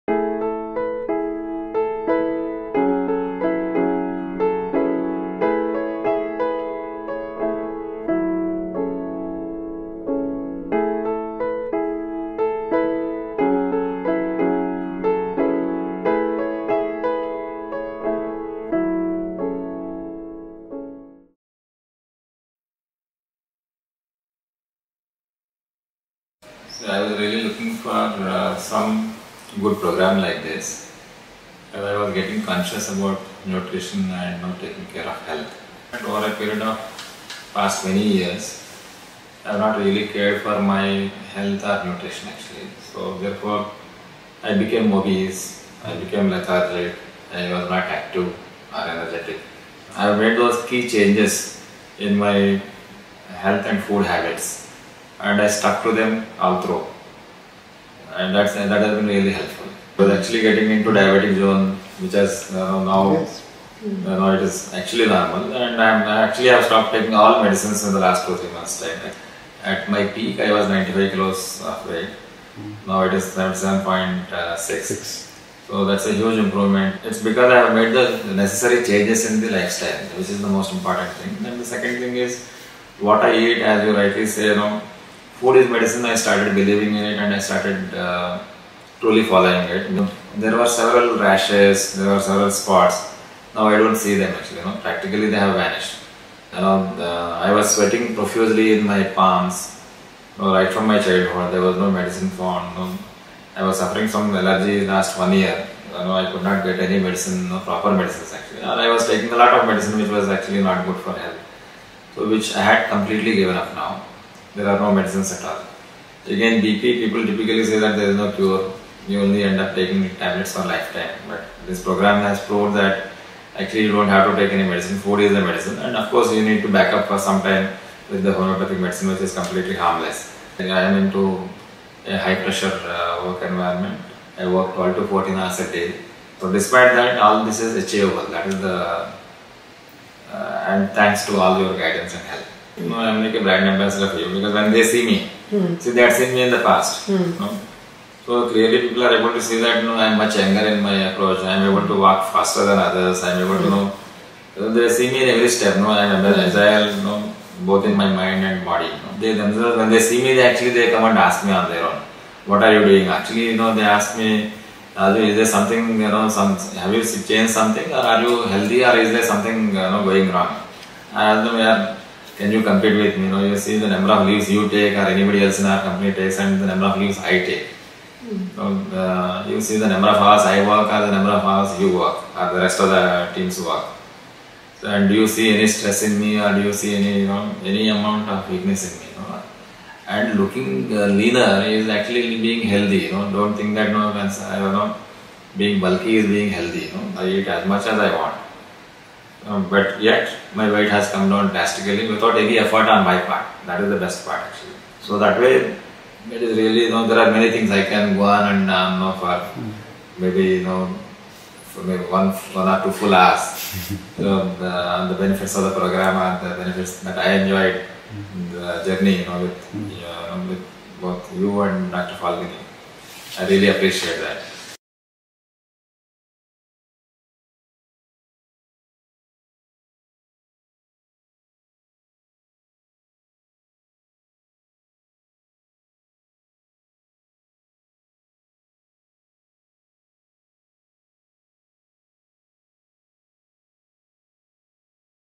A case of Hypertension (high B. P.), Hyperlipidemia (high cholesterol, triglycerides) & Obesity
In his quest ‘for a good doctor who could guide him on nutrition and lifestyle so that he can enjoy super health and life’, he came to CHRC for consultation.

He was obese. He was suffering from High B.P. since 2002. He was taking Ciplar LA 40 and Telma 20 for high BP. Also, he was suffering from urticaria (allergic rash) since 1 year. He was prediabetic. On investing him further, his lipid profile was highly abnormal.
His blood test reports were as follows.

Lipid profile: Cholesterol (Total )  : 221 mg/dl
                        Triglycerides             : 230 mg/dl
                        VLDL                          : 46 mg/dl
Serum Homocystein : 4.00 ug/ml
Total IgE: 332    (Normal: 0 – 91)

He joined NEST – Lifestyle Disorder Reversal Program. His blood tests at the end of the Program reflected the betterment he was experiencing.
Lipid profile: Total  Cholesterol     : 168 mg/dl
                        Triglycerides             :   96 mg/dl
                        VLDL                          :   19 mg/dl
                        Serum Homocystein : 2.50 ug/ml
                        Total IgE                    : 109  (Normal: 0 – 91)

Since coming to CHRC, he has lost 17.4 kgs weight. His lipid profile has become normal and he is off all allopathic tablets. His total IgE has dropped from 332 to 109 which is approaching normalcy.